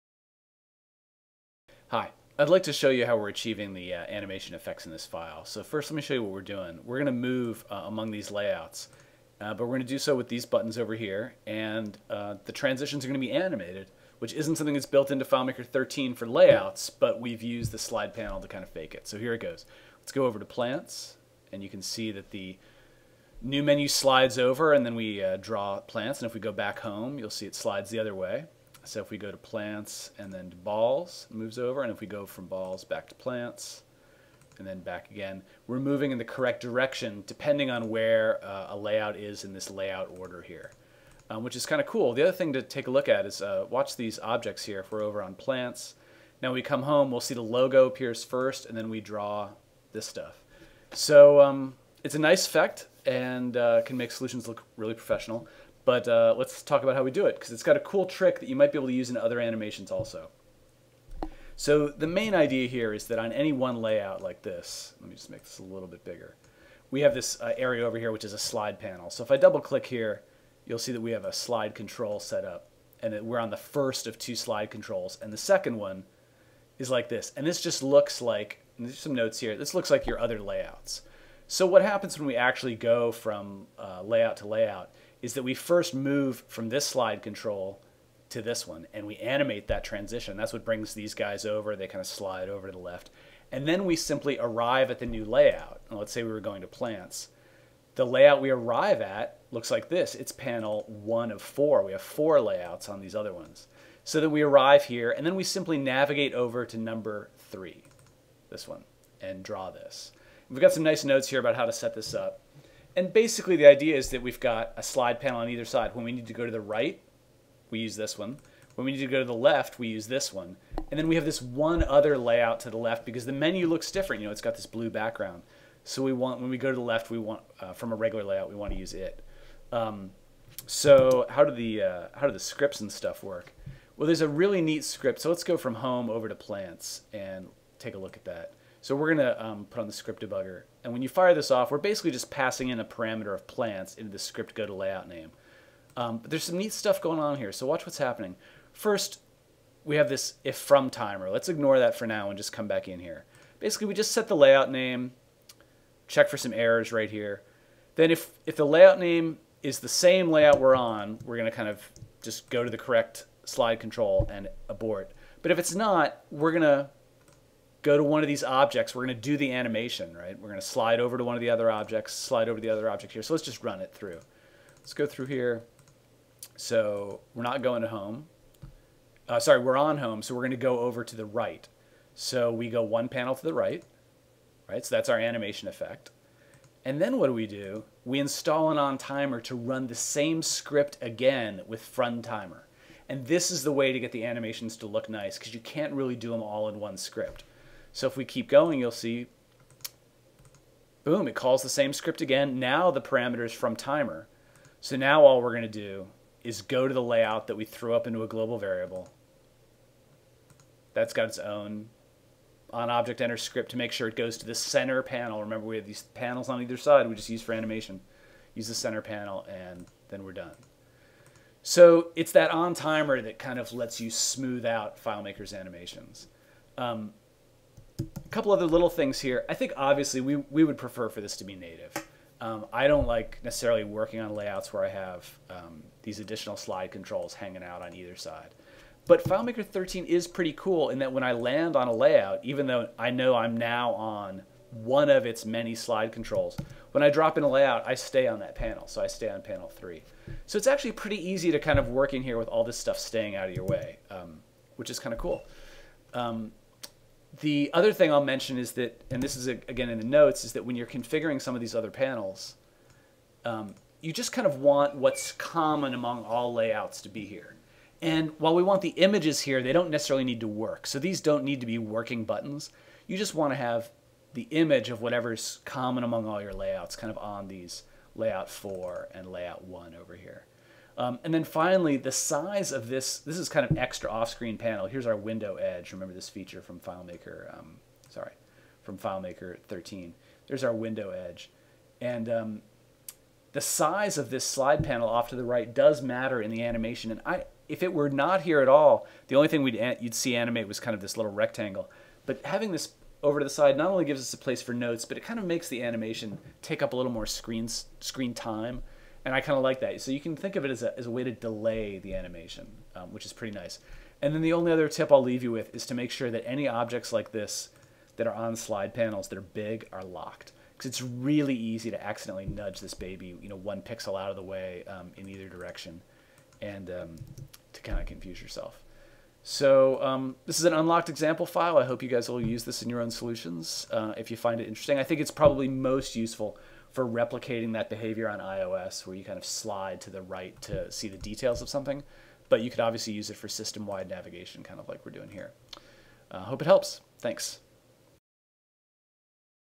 FileMaker 13 Go To Layout Animation
The animations in FileMaker 13 are awesome, but there is no way to animate the transition from one layout to another... or is there?

The unlocked example file shown here (available walks you through how to do this and introduces a technique for smoothing our the animation of slide panels within scripts.